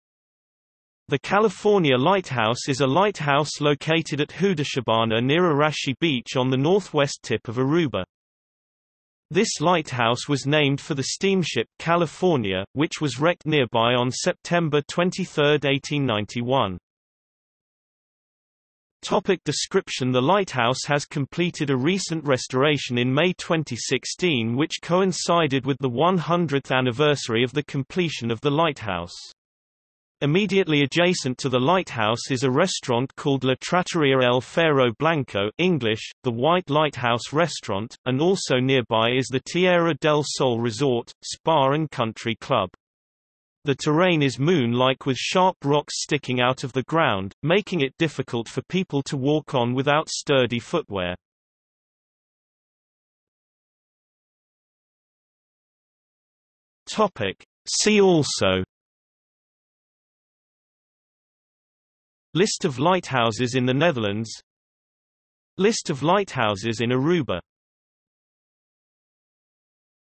California Lighthouse | Wikipedia audio article
00:00:18 Description
00:00:53 See also

- Socrates

SUMMARY
The California Lighthouse is a lighthouse located at Hudishibana near Arashi Beach on the northwest tip of Aruba.
This lighthouse was named for the steamship California, which was wrecked nearby on September 23, 1891.